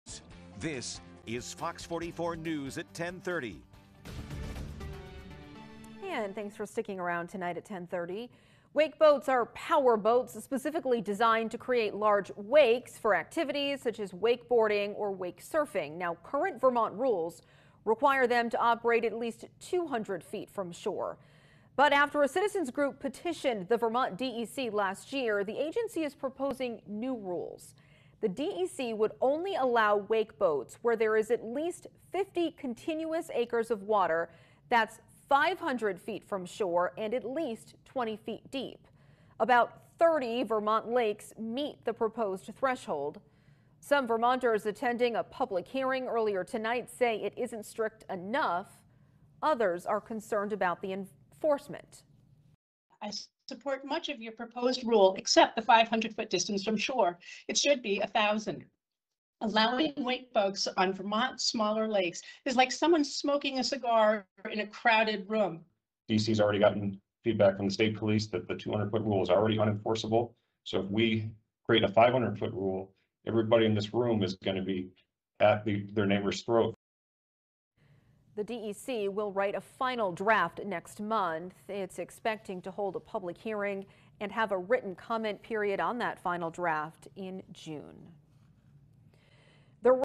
Vermont DEC wants to tighten rules for wake boats
A citizens' group petitioned the DEC last year about its wake boat rules. That group doesn't think the agency's proposal goes far enough.